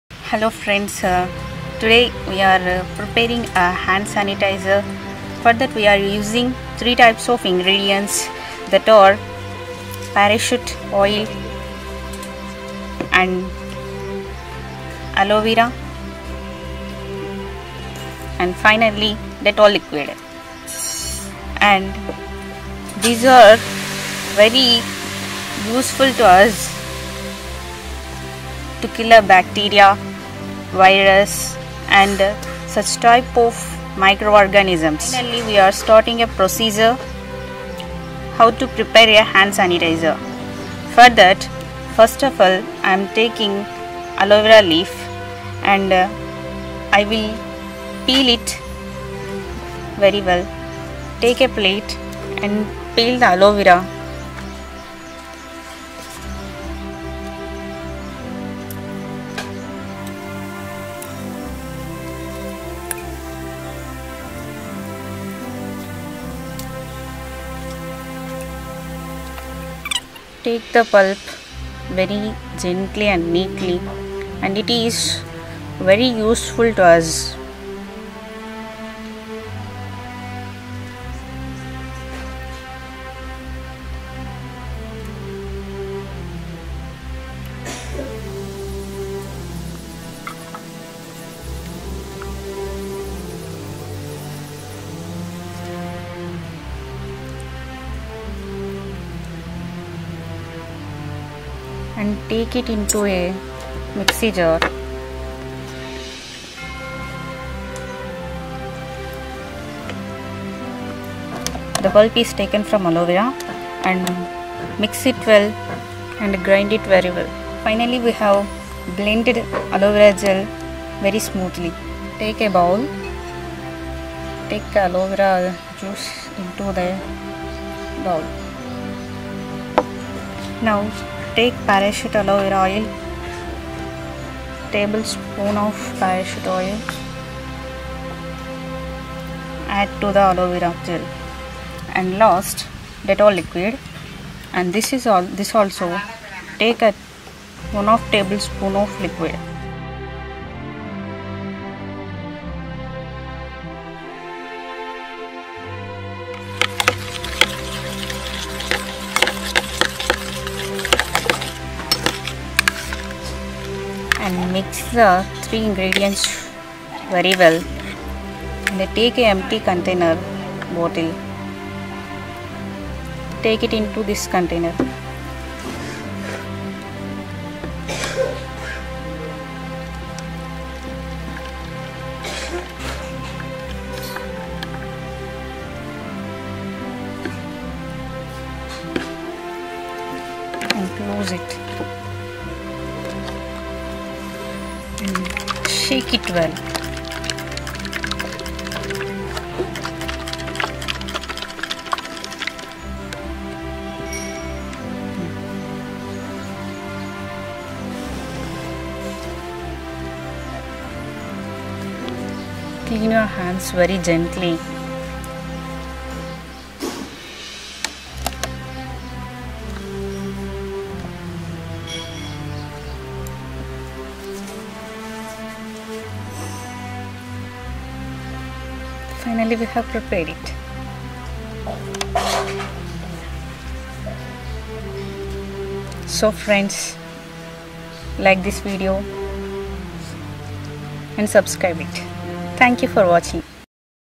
How to make Hand Sanitizer at home Using Dettol Liquid And Aloe Vera | homemade Hand Sanitizer
How to make Hand Sanitizer at home Using Dettol Liquid And Aloe Vera Gel | DIY Hand Sanitizer